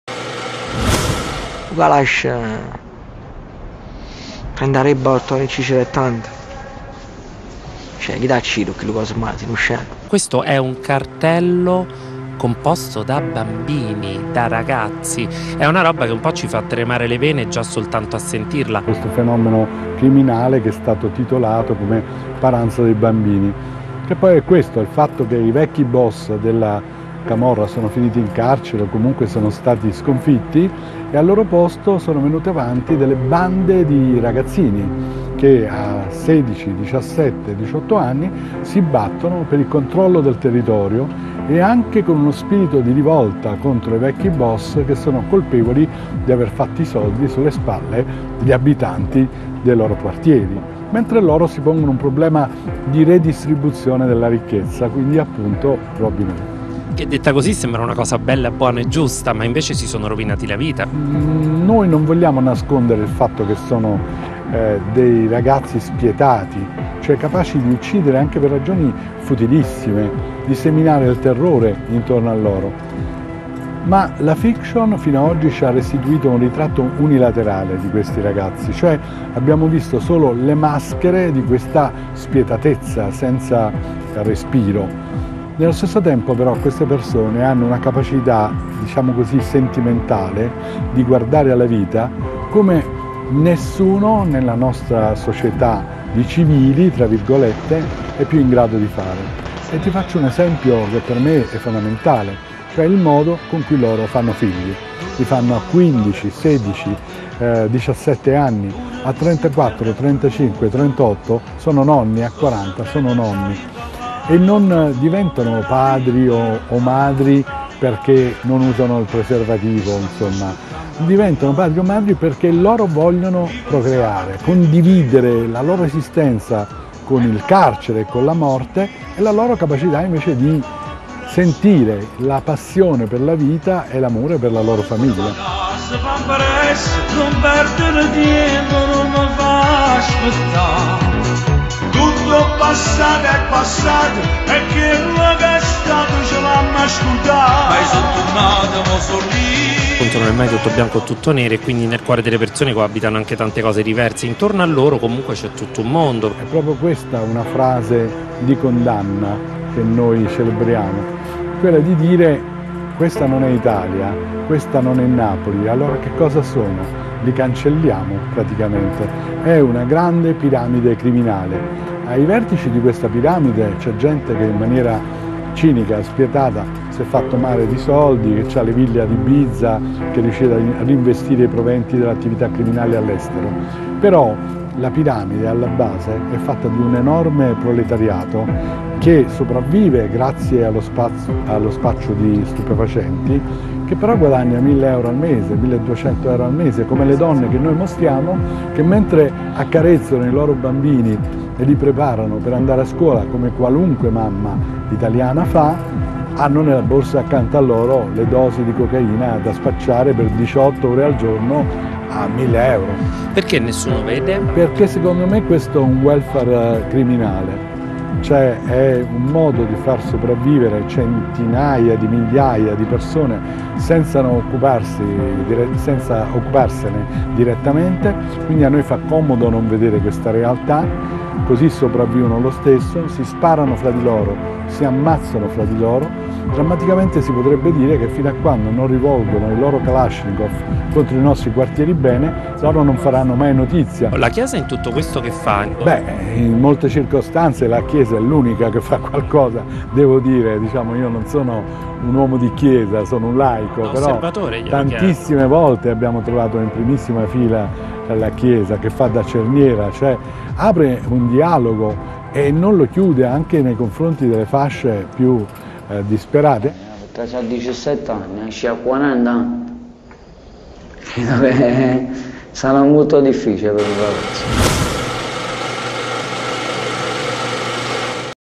Michele Santoro presenta Robinù al Festival di Venezia
Michele Santoro racconta a Fabio Falzone il suo docufilm “Robinu’”: la camorra, i giovanissimi, una rete di criminalità che le istituzioni preferiscono non vedere, la Chiesa da sola in trincea.